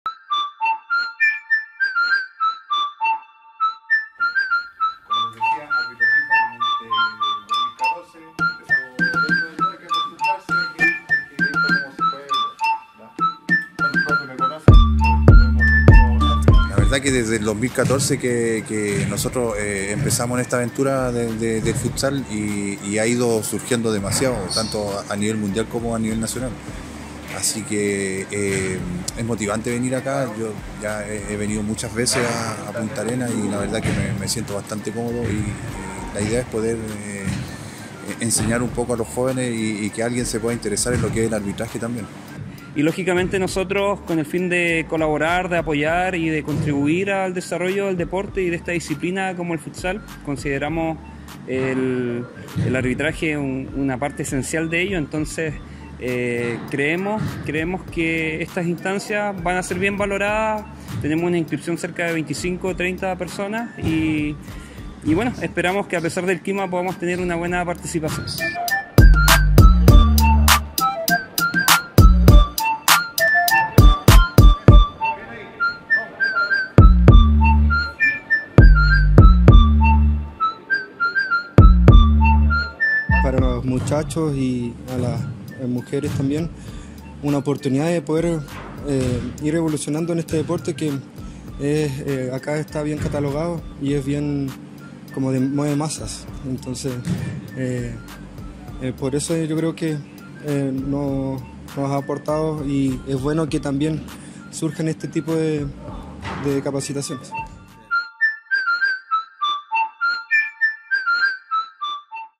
Juez FIFA de la ANFP compartió capacitación para árbitros de futsal de Magallanes
Durante tres días el experimentado juez FIFA de la ANFP Cristian Espíndola ofreció una capacitación en el gimnasio universitario para árbitros de futsal de Magallanes.

Las jornadas generaron gran interés y fueron organizadas por la Asociación de Árbitros Independientes de Magallanes y el Área de Deportes UMAG.